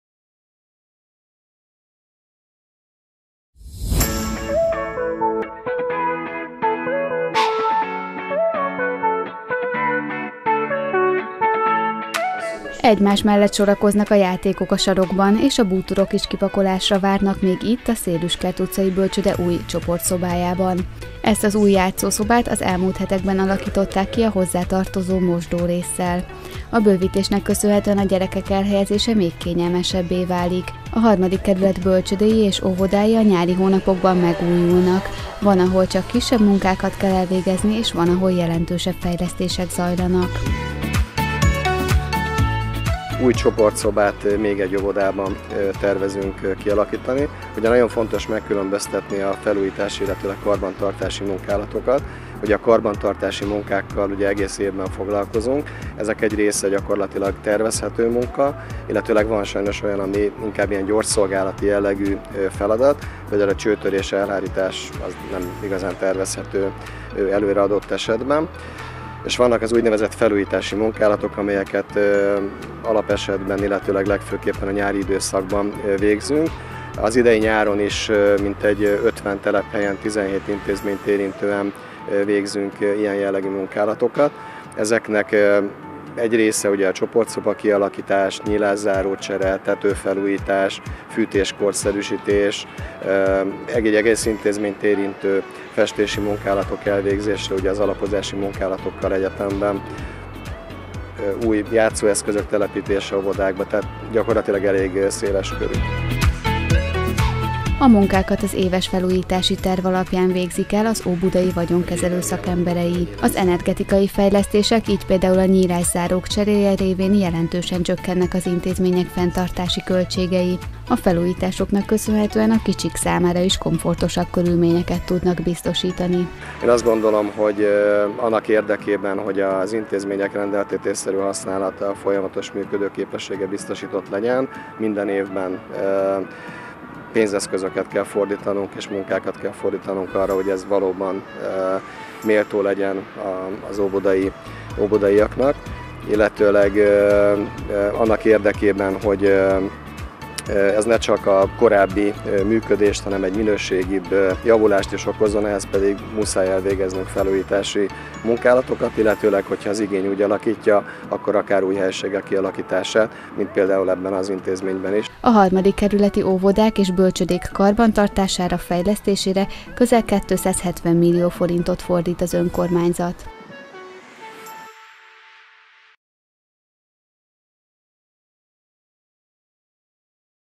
Felújítások az óbudai bölcsődékben és óvodákban
A nyár a felújítások időszaka az Óbudai Egyesített Bölcsődék intézményeiben és az óvodákban is. A fejlesztési munkálatokat az Óbudai Vagyonkezelő Nonprofit Zrt. végzi, Óbuda-Békásmegyer Önkormányzata közel 370 millió forintot fordít a kerületben található található bölcsődék és óvodák fejlesztésére.
Óbuda - közösséget építünk